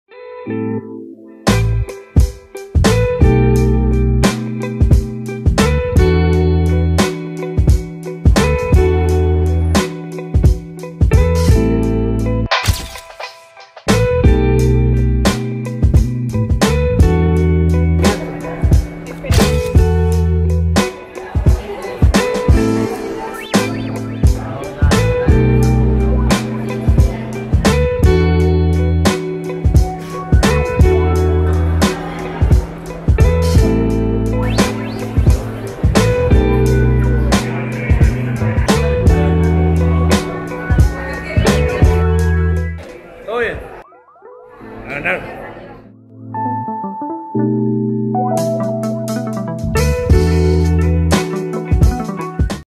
[Plantel Profesional ⚽] 🔴🔵 ¿Todo bien? A ganar... 🤙
El Poderoso ya está en Ibagué para disputar la primera fecha del Campeonato de Primera División 2020-I. 👏 VamosMedellín | Siempre con vos.